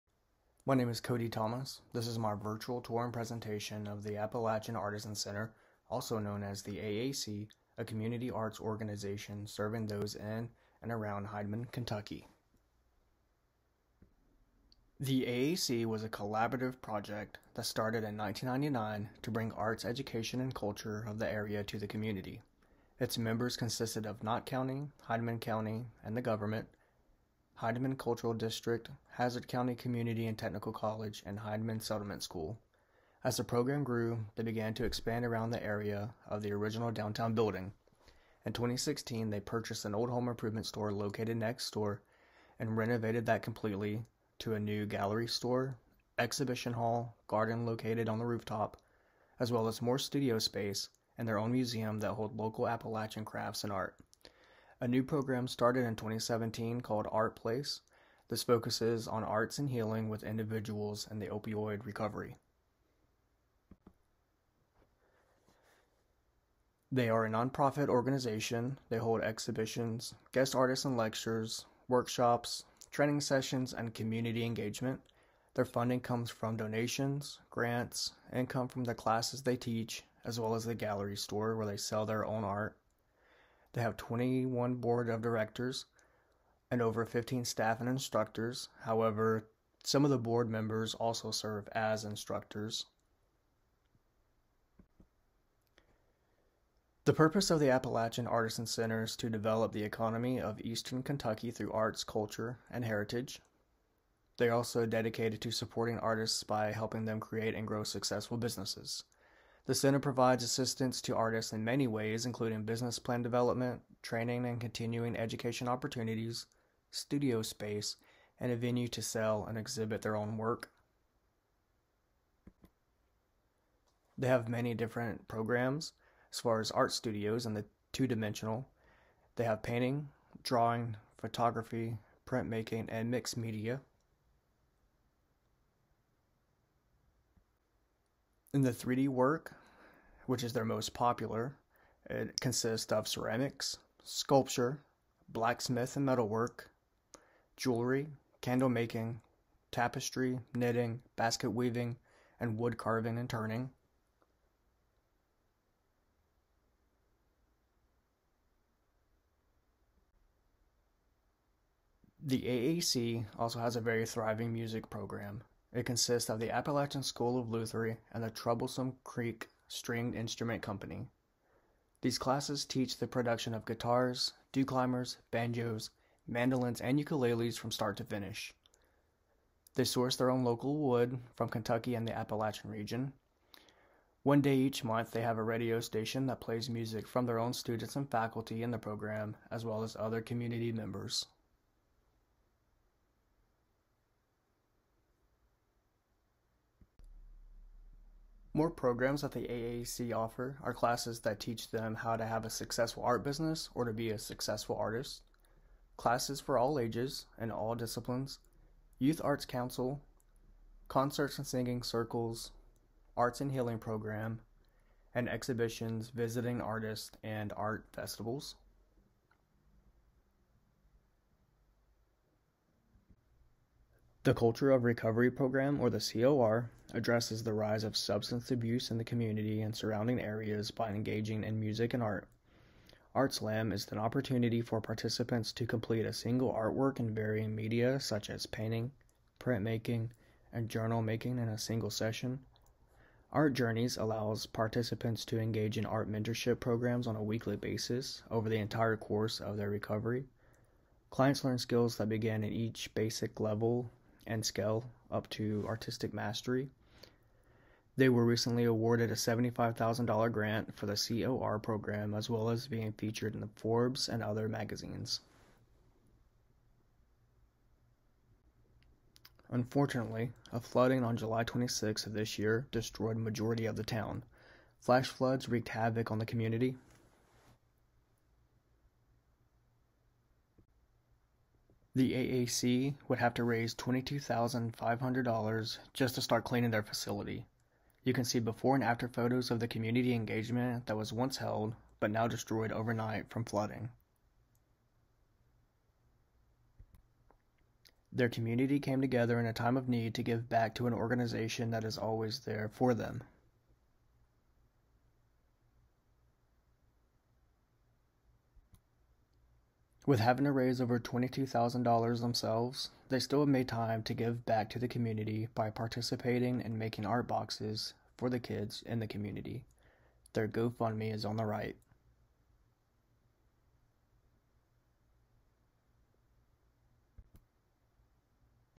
Appalachian Artisan Center: Virtual Tour & Presentation
ART 6933: Art Education Beyond School Walls
UFL Art Ed Master's Program
1 September 2022